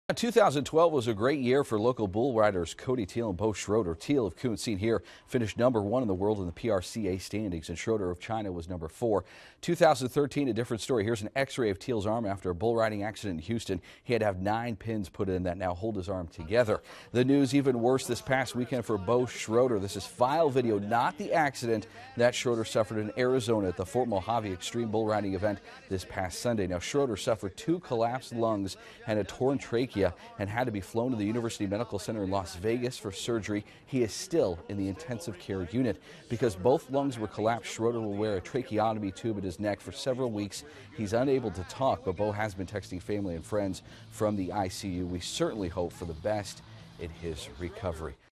Local bull rider injured
KFDM News - Southeast Texas bull rider Beau Schroeder suffered injuries in a bull riding event in Arizona this past Sunday.He was competing in the Fort Mohave Extreme Bull Riding event. Schroeder, who's from China, suffered two collapsed lungs and a torn trachea and had to be flown to the University Medical Center in Las Vegas for surgery.He was still in the intensive care unit on Tuesday afternoon.Because both lungs were collapsed, Schroeder will wear a tracheotomy tube in his neck for several weeks and he's unable to talk.He has been texting family and friends from the ICU.